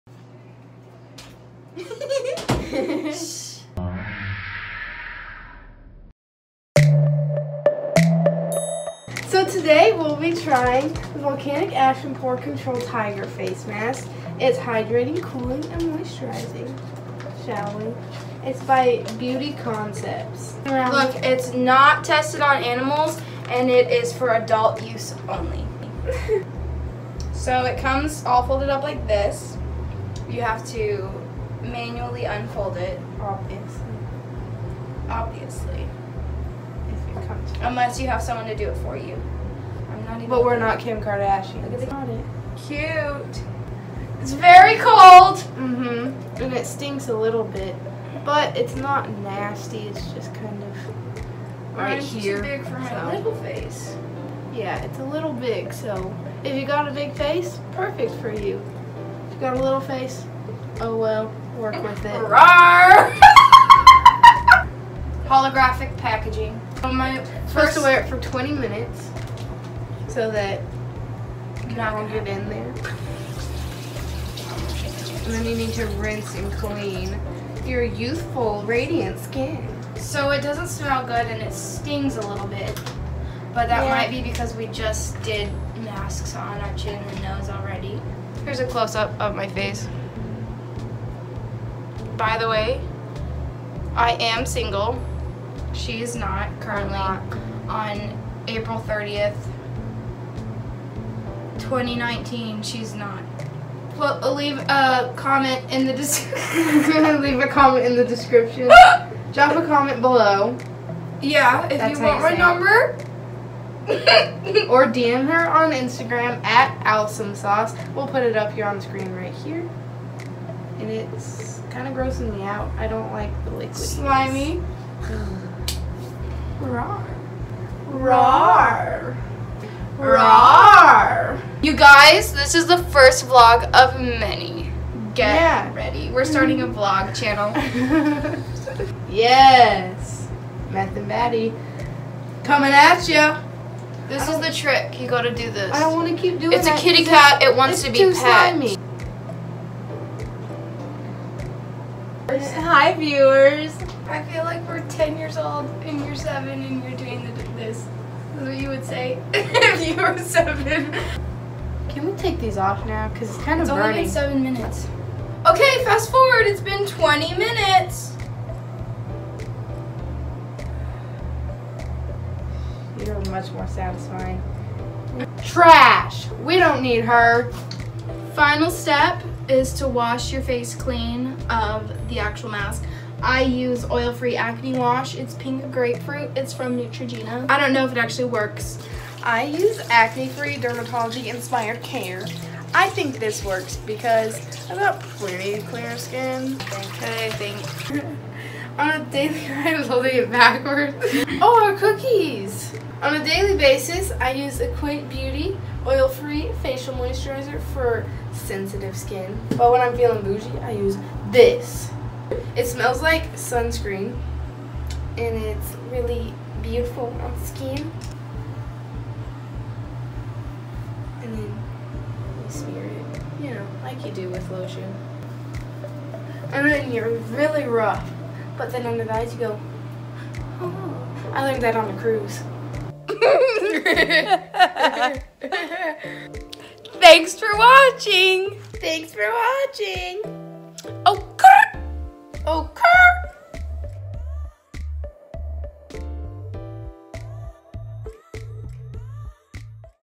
We Tried Tiger Face Masks || Vlog 1
We tried volcanic ash tiger face masks and decided that we were gonna start vlogging.